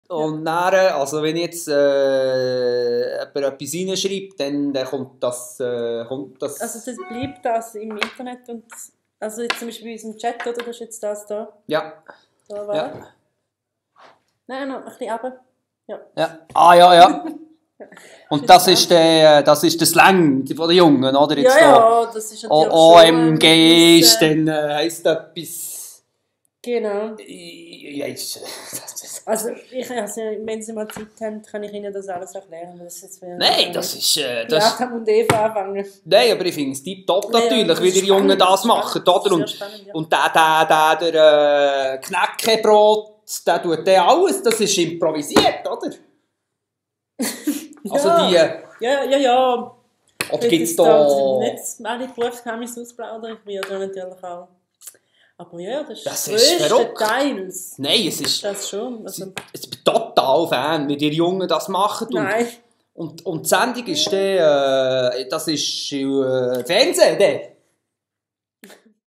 Ernst Wüst zu Besuch bei joiz
wir haben einen neuen senior-praktikanten.
er lernt nach und nach das ganze joiz-team kennen.
erstes opfer: knackattack-produzentin LYA